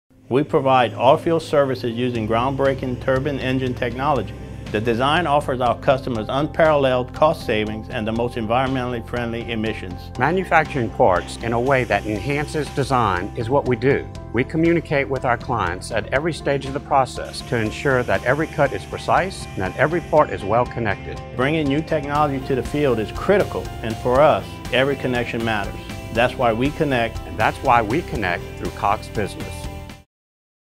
Green Field Energy & Begneaud Manufacturing
Cox Business customer testimonial spot featuring the strengths of Green Field Energy Services and Begneaud Manufacturing and how the two businesses work together using services from Cox Business. Spot features Jerry Broussard and Don Begneaud.